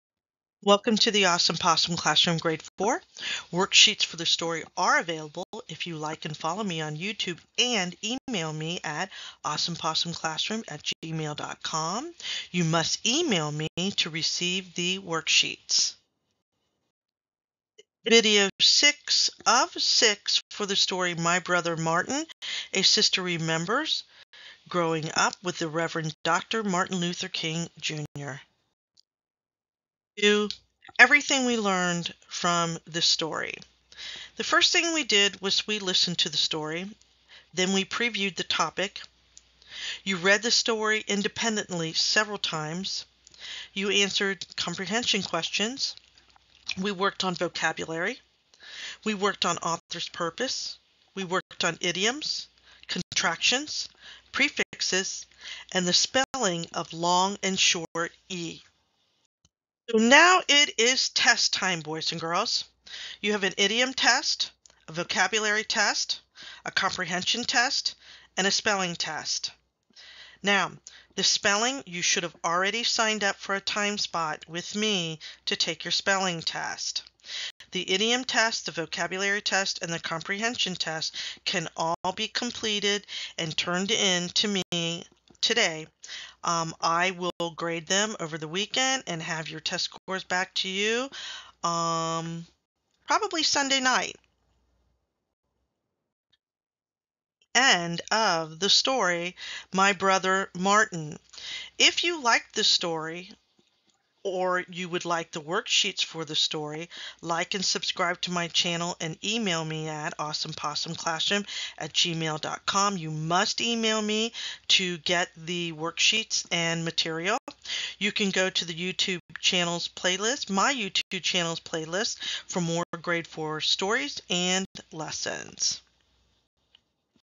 My Brother Martin Video 6 End of Story Review
This is a  6-video series of lessons for the story My Brother Martin, A Sister Remembers Growing Up With the Rev. Dr. Martin Luther King Jr., from the Journeys Grade 4 reading series.
GREAT FOR HOMESCHOOL
• Read along
• Authors Purpose
• Contractions
• Spelling
• Prefixes
• Vocabulary
• Comprehension questions
• Assessment
To view all videos for My Brother Martin, go and search the playlist.  Additional grade 4 stories are being added.